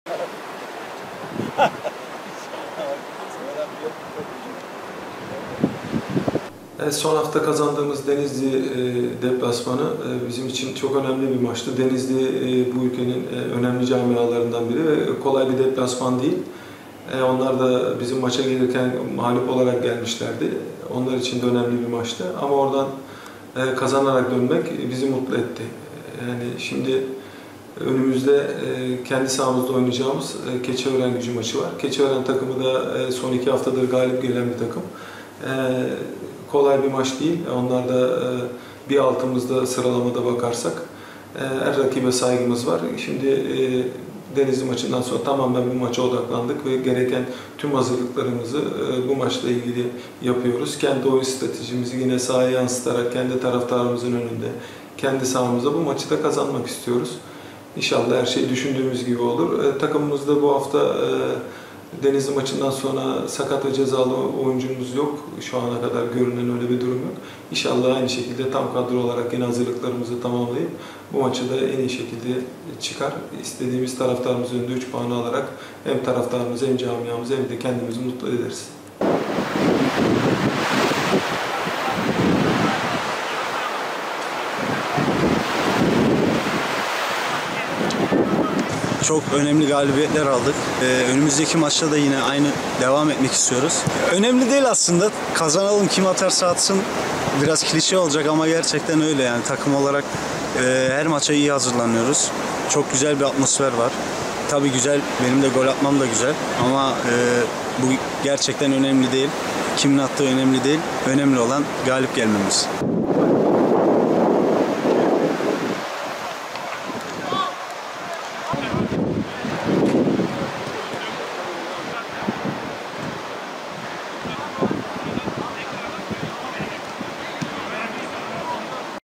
Bandırmaspor galibiyet serisini sürdürmek istiyor | Bandirma.com
Spor Toto 1. Lig'de geride kalan 4 haftada çıktığı 3 maçta 9 puan toplayarak ikinci sırada yer alan Beyçimento Bandırmaspor, Ankara Keçiörengücü maçına da galibiyet hedefiyle hazırlanıyor.